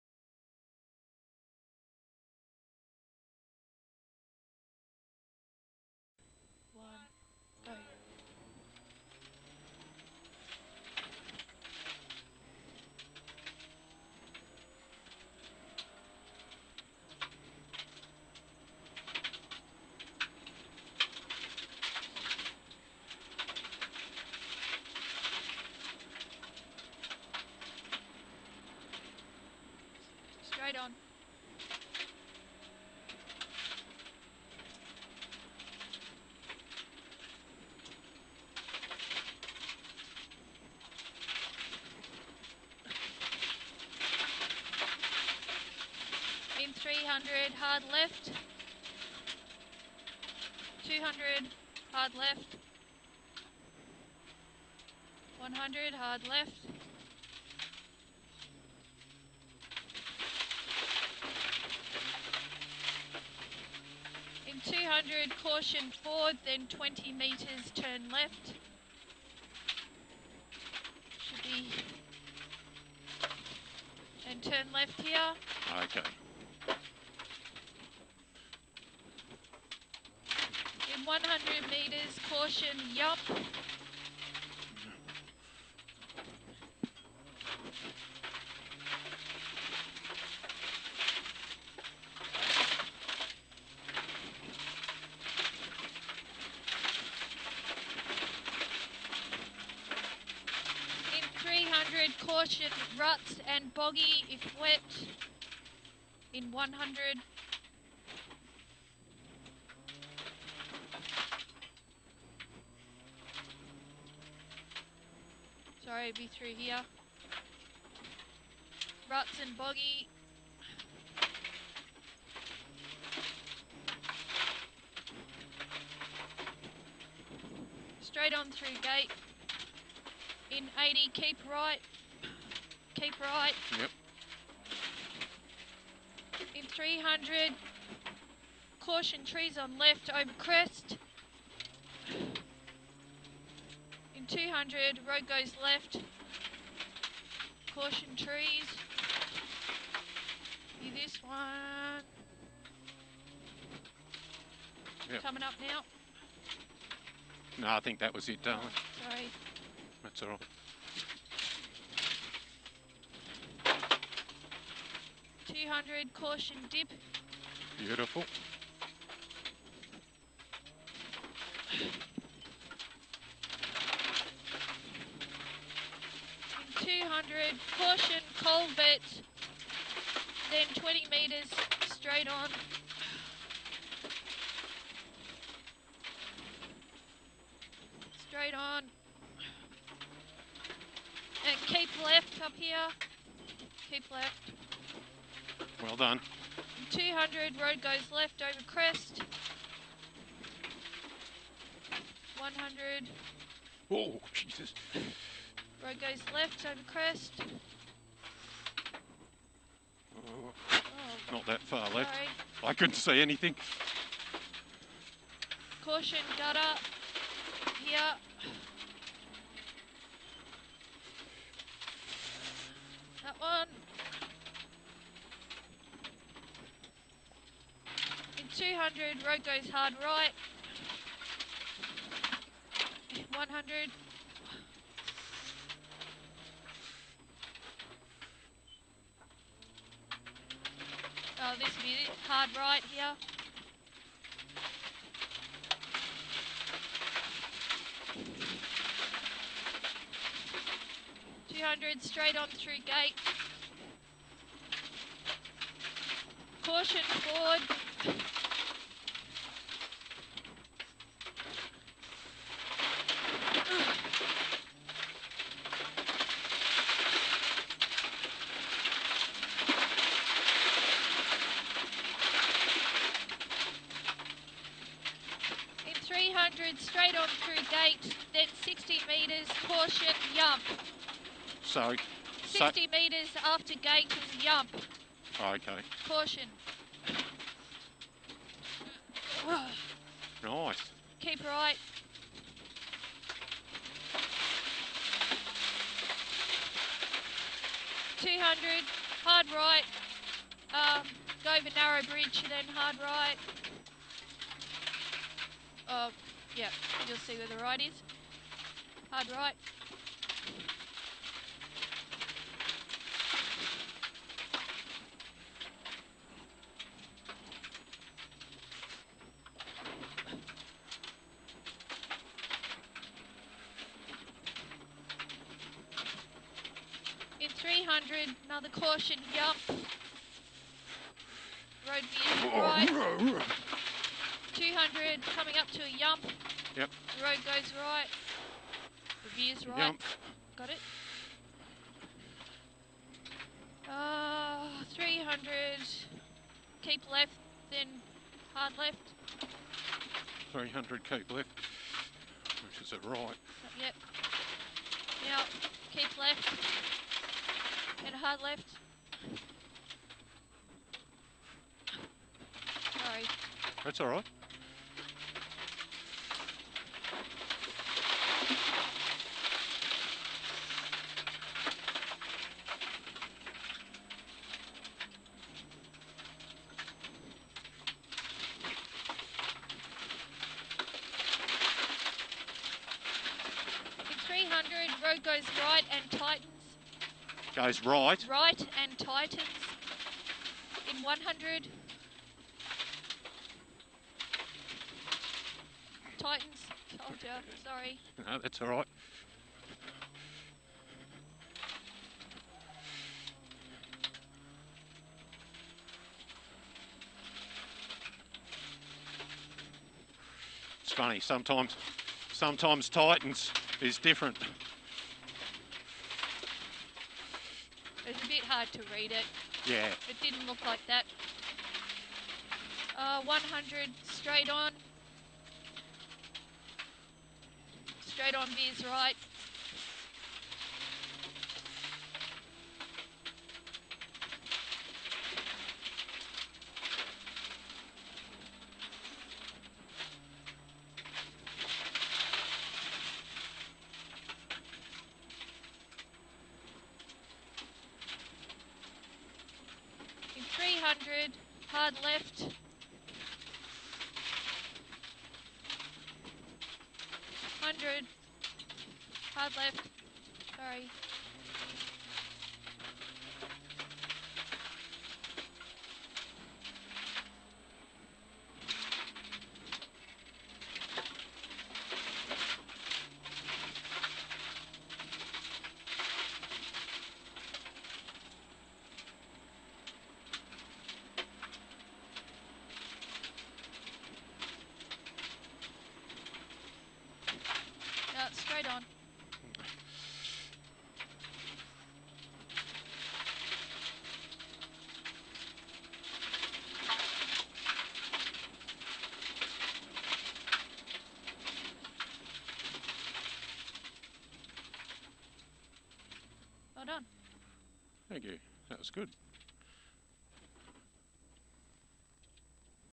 George Derrick 2009 Stage 5, Hyundai X3 Excel (Accent and Pony)
Stage 5 of the 2009 George Derrick Rally based around Avoca and Mount Cole in Central Victoria.
Not a star in a reasonably priced car! One make rally series Hyundai Excel (Accent or Pony in some countries). Cars run standard engines, gear boxes and brakes, only mods are suspension and safety equip